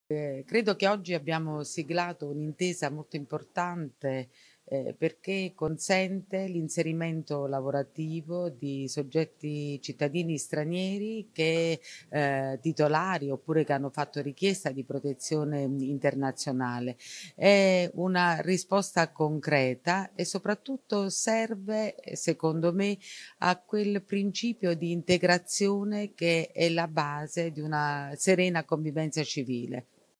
Intervento ministro Lamorgese: firma protocollo per l’inserimento di cittadini stranieri vulnerabili
Roma, ministero del Lavoro e delle Politiche Sociali dove ha sottoscritto - insieme al ministro Orlando, al presidente dell’Associazione Nazionale Costruttori Edili Gabriele Buia e i rappresentanti delle organizzazioni sindacali - il protocollo d’intesa per favorire l’insediamento socio lavorativo di richiedenti e titolari di protezione internazionale e altri cittadini stranieri in condizione di vulnerabilità.
Fonte: ministero del Lavoro e delle Politiche Sociali